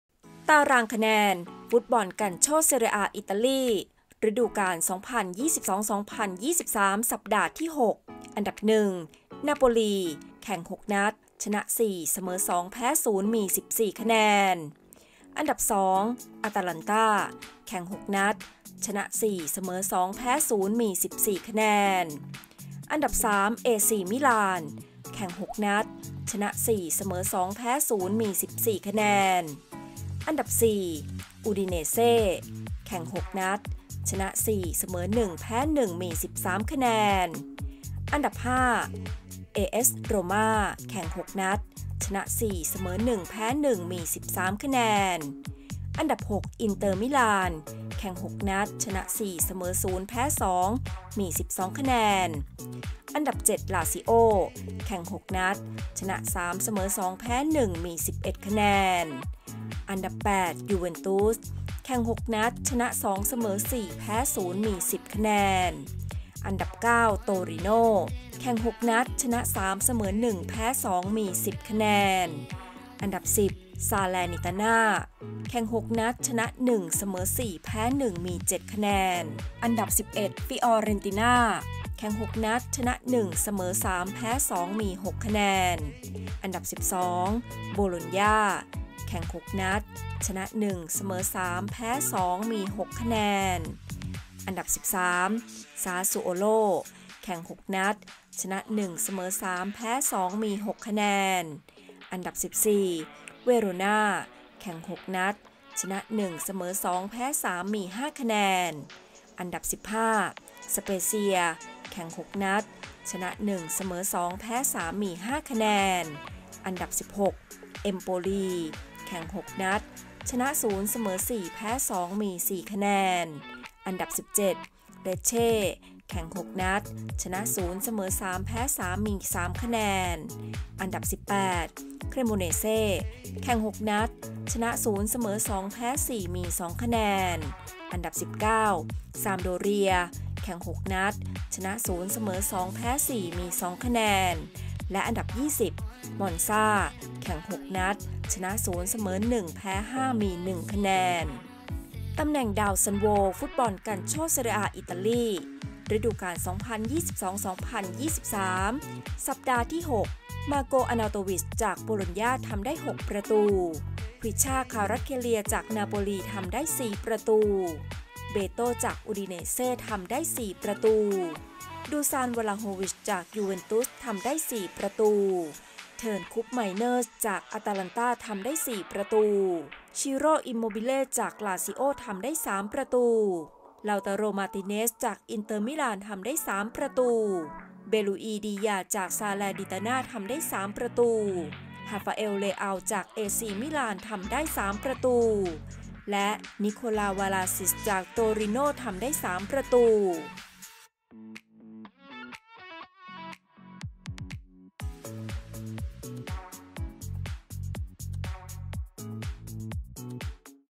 สรุปตารางคะแนน+ดาวซัลโว ฟุตบอลกัลโช่เซเรียอาอิตาลี ล่าสุด (นัดที่ 6 |สัปดาห์ที่ 6)
ตารางคะแนน ฟุตบอลกัลโช่เซเรียอา อิตาลี
อันดับที่ 1 - นาโปลี   ||แข่ง 6 นัด|| ชนะ 4 | เสมอ 2 | แพ้ 0 || มี 14 คะแนน
อันดับที่ 2 - อตาลันต้า ||แข่ง 6 นัด|| ชนะ 4 | เสมอ 2 | แพ้ 0 || มี 14 คะแนน
อันดับที่ 3 - เอซี มิลาน ||แข่ง 6 นัด|| ชนะ 4 | เสมอ 2 | แพ้ 0 || มี 14 คะแนน
อันดับที่ 4 - อูดิเนเซ่ ||แข่ง 6 นัด|| ชนะ 4 | เสมอ 1 | แพ้ 1 || มี 13 คะแนน
อันดับที่ 5 - เอเอส โรม่า ||แข่ง 6 นัด|| ชนะ 4 | เสมอ 1 | แพ้ 1 || มี 13 คะแนน

ตำแหน่งดาวซัลโว ฟุตบอลกัลโช่เซเรียอา อิตาลี
อันดับที่ 1 - มาร์โก อเนาโตวิช - โบโลญญ่า
อันดับที่ 2 - ควิซ่า คารัตส์เคเลีย - นาโปลี
อันดับที่ 2 - เบโต้ - อูดิเนเซ่
อันดับที่ 3 - ดูซาน วลาโฮวิช - ยูเวนตุส
อันดับที่ 4 - เทิน คูปไมเนอร์ส - อตาลันต้า